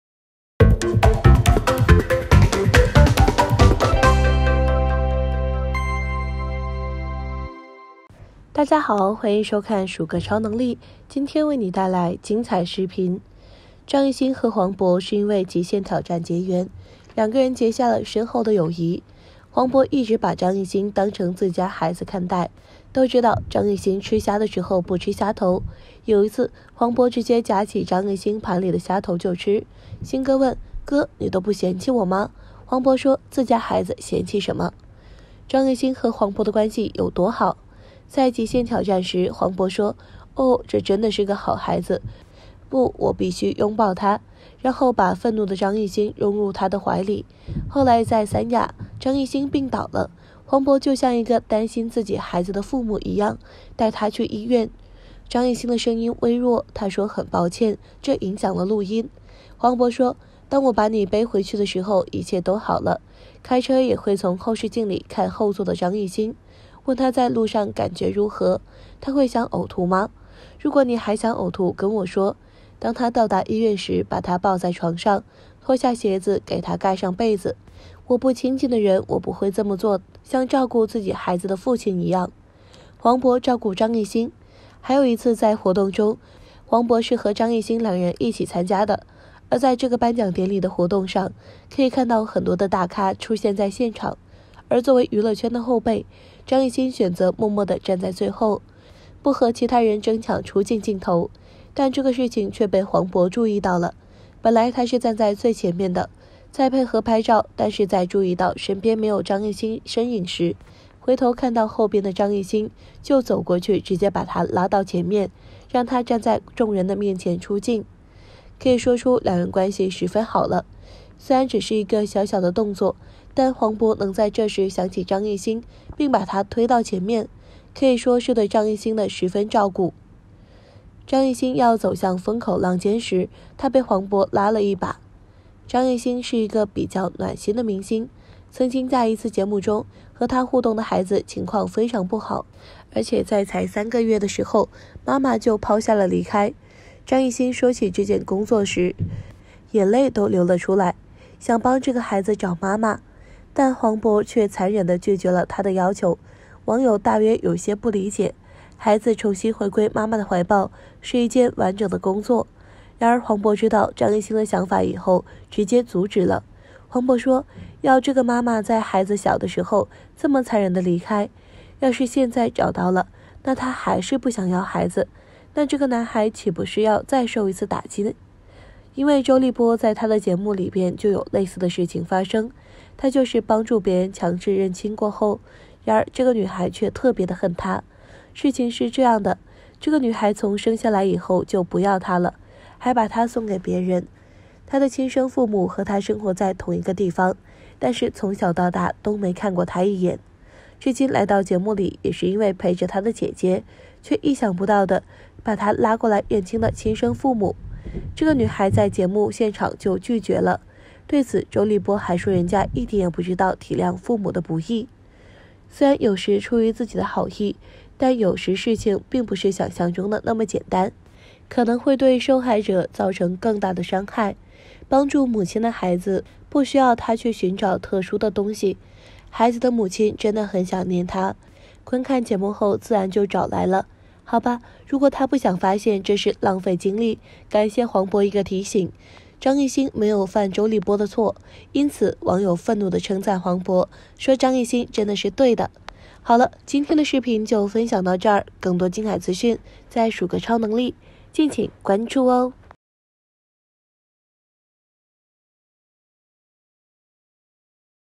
張藝興做錯了什么？多虧黃渤奮不顧身的幫一把，張藝興才沒重蹈周立波的覆轍 !數個超能力
張藝興做錯了什么？多虧黃渤奮不顧身的幫一把，張藝興才沒重蹈周立波的覆轍 !數個超能力#張藝興#黃渤#周立波